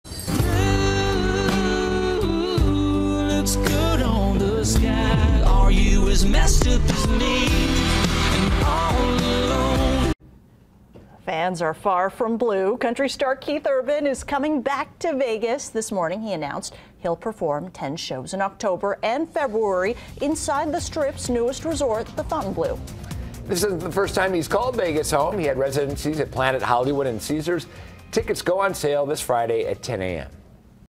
Keith Urban announces Fontainebleau engagement
The country star's 10-show residency will begin on October 4.